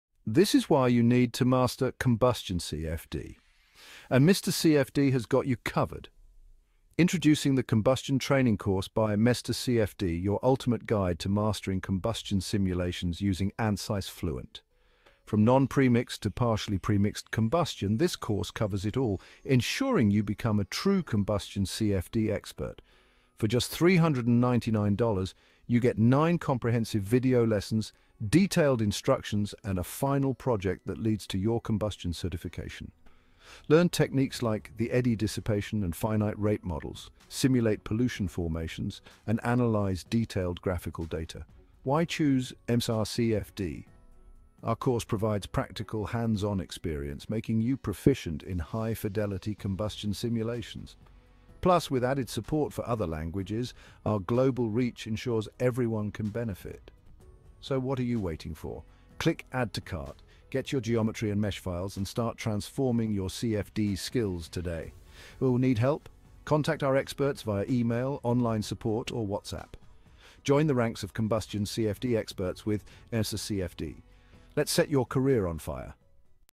Combustion Training Course BY ANSYS FLUENT
Are you looking to become a Combustion CFD Simulation expert? Look no further! MR CFD is proud to offer our comprehensive Combustion Training Course using ANSYS Fluent.

🎓 What You’ll Learn:

    Introduction to combustion and CFD simulation techniques
    Non-premixed and premixed combustion modeling
    Pollution formation simulation
    Advanced combustion models including PDF transport

💡 Key Features:

    9 in-depth video lessons
    Final project simulation exam
    Combustion Certification upon completion
    Geometry and mesh files included
    Comprehensive ANSYS Fluent training video

Whether you’re an academic researcher or industry professional, this course will elevate your combustion simulation skills to the next level.